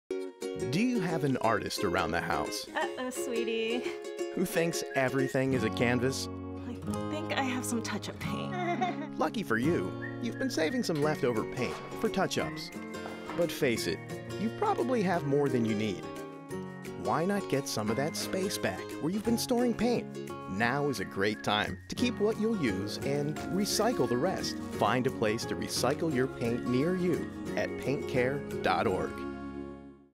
Keep What You Need and Recycle the Rest!
Why not get some of that space back where you’ve been storing paint? Now is a great time to keep what you’ll use, and recycle the rest. Find a drop-off location near you at paintcare.org.